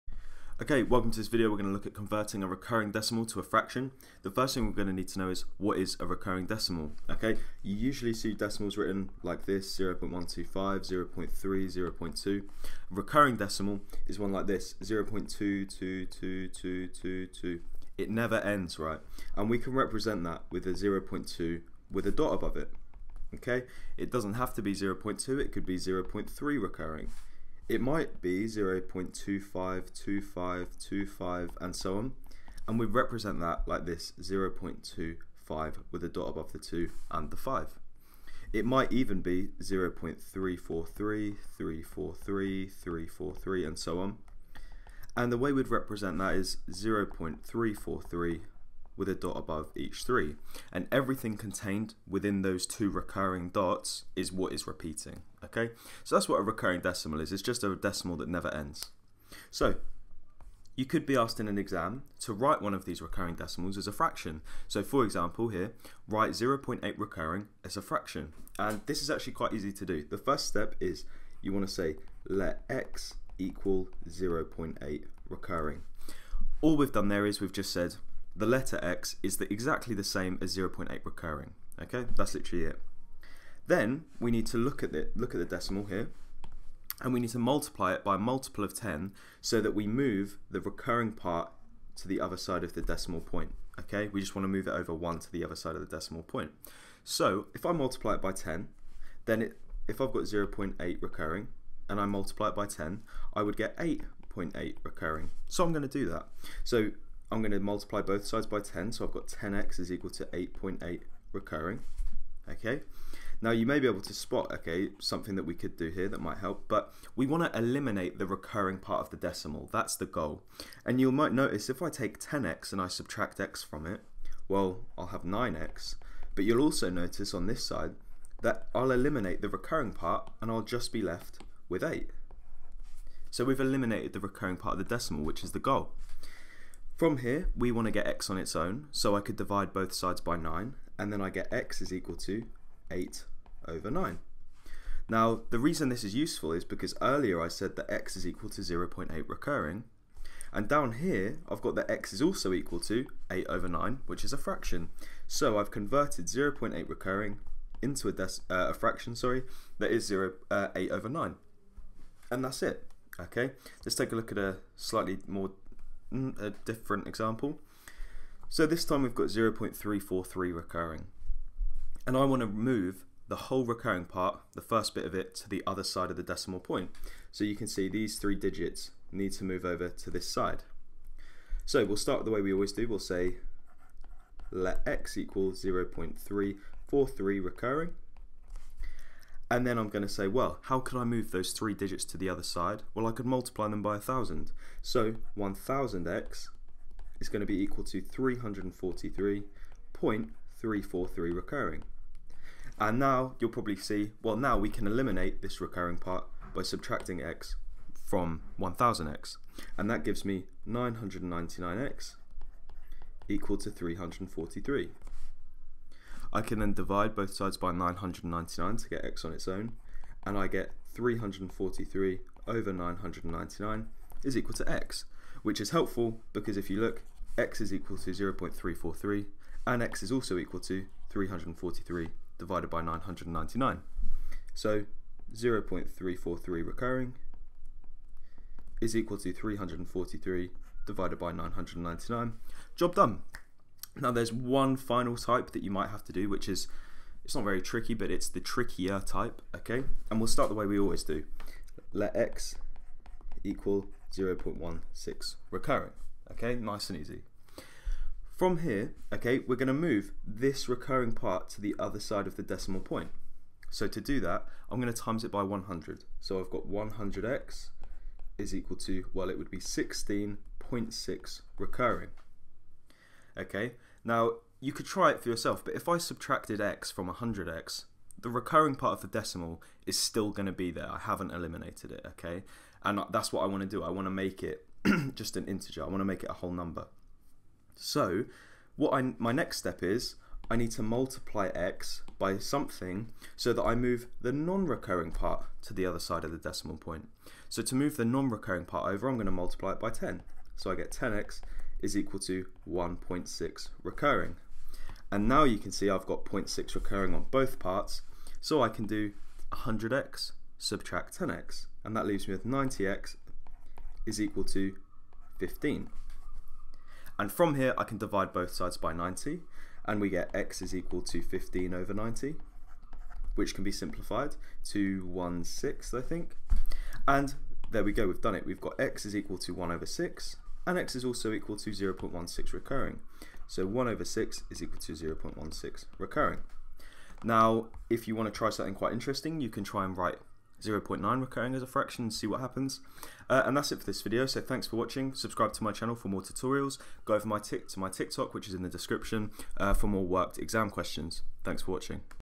Writing Recurring Decimals as Fractions | GCSE Maths
In this GCSE Maths video I will show you how to write a recurring decimal as a fraction. I give a few examples covering all the things you might come across in your exam. This can look tricky, but its actually really easy todo!